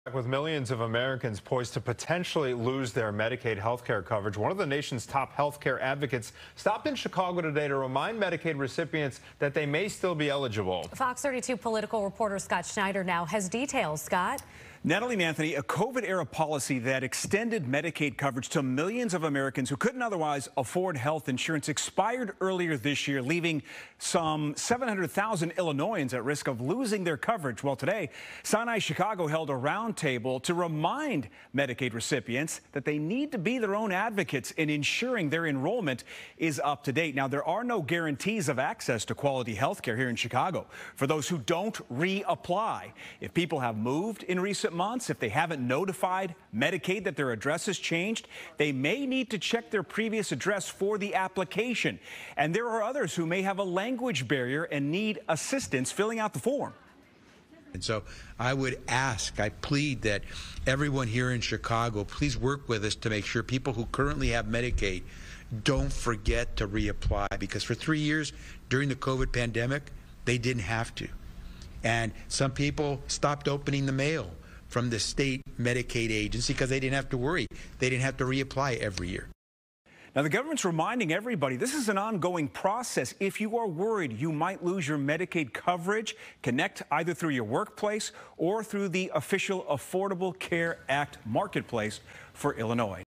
Medicaid renewal process: Thousands of Illinoisans at risk of losing coverage
This year alone, as many as 15 million Americans could potentially lose their Medicaid health care coverage. One of the nation's top health care advocates made a stop in Chicago on Monday to remind Medicaid recipients that they may still be eligible.